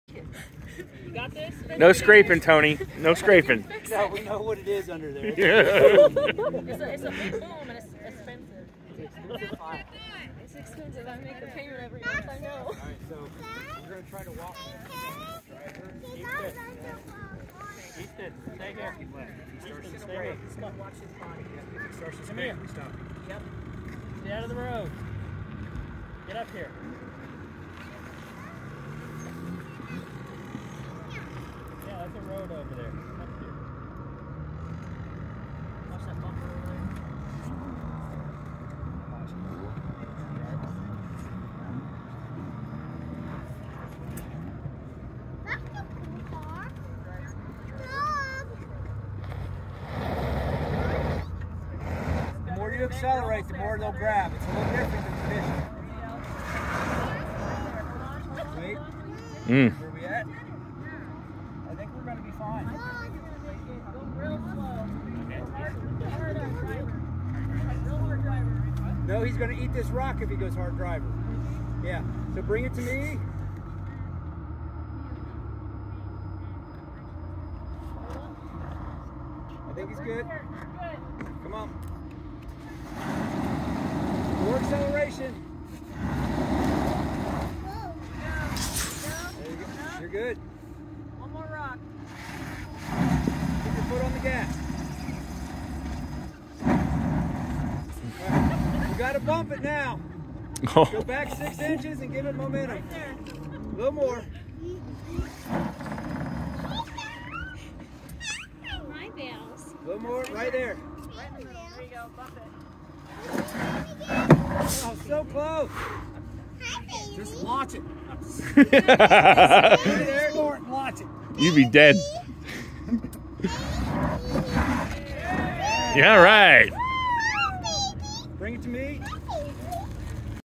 Rivian rock climbing (short)
Here's a short clip of a Rivian climbing over a 3ft rock ledge in an off-road class on 11/12/2022 in Colorado Springs!